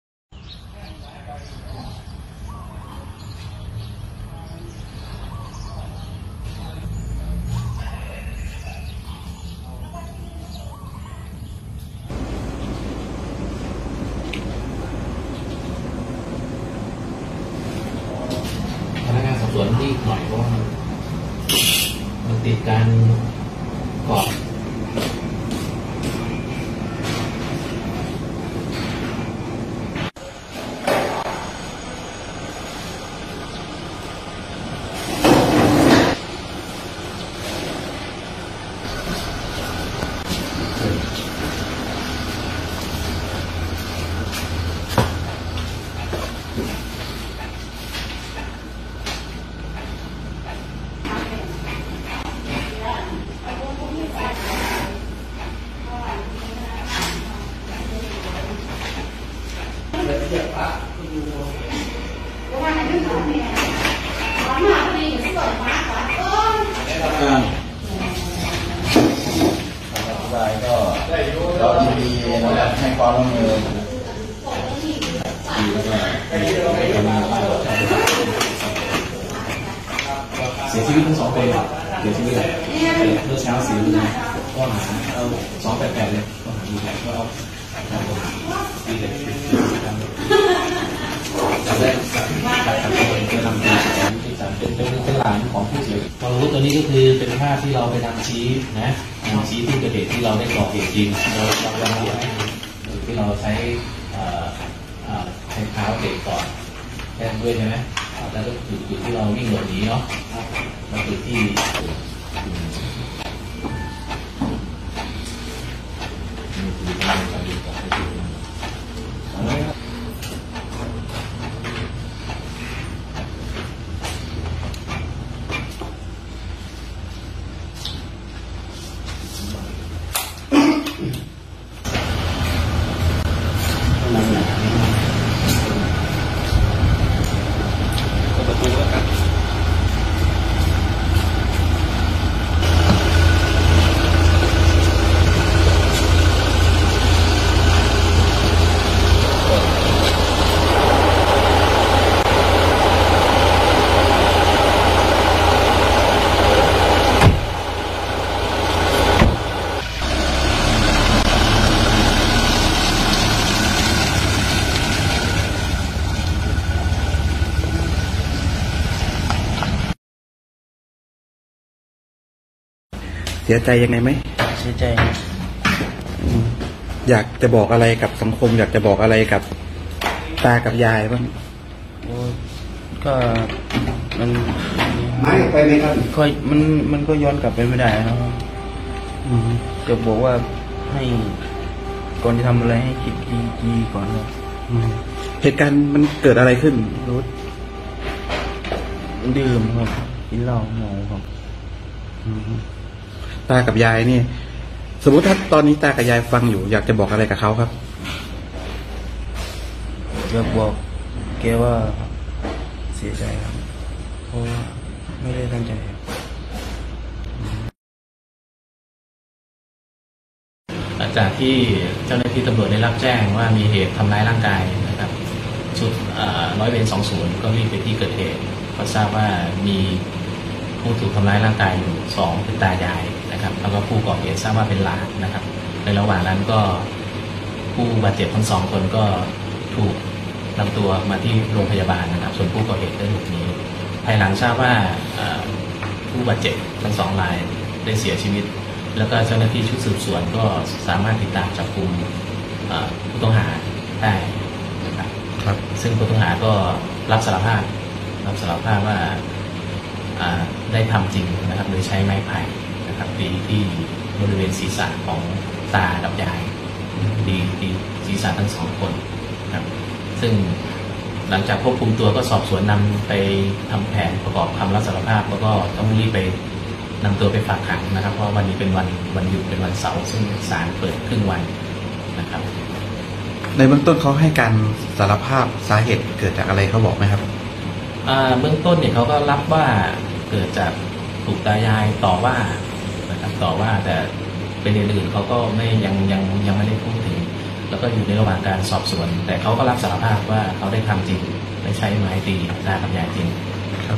รวบหลานทรพี!เมาเหล้า-ยาบ้า ใช้ไม้ตีตา-ยายดับ อ้างโมโหถูกอบรมให้เป็นคนดี
รวบหลานทรพี!เมาเหล้า-ยาบ้า ใช้ไม้ตีตา-ยายดับ อ้างโมโหถูกอบรมให้เป็นคนดี 16/11/2019